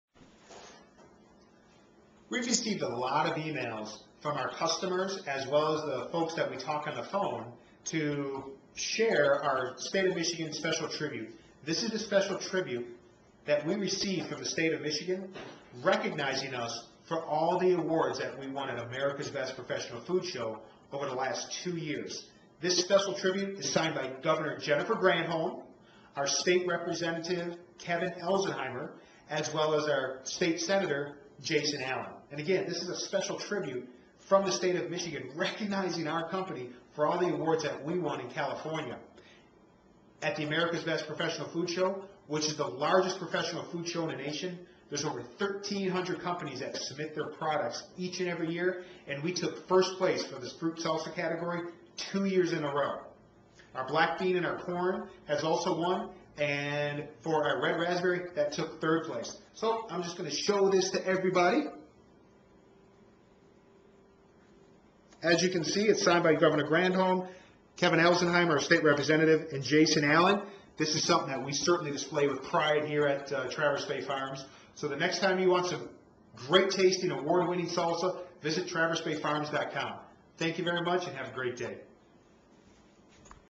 Award Winning Salsas Receive Special Tribute Award - Traverse Bay Farms
Award Winning Salsas from Traverse Bay Farms receive Special Tribute Award from the State of Michigan.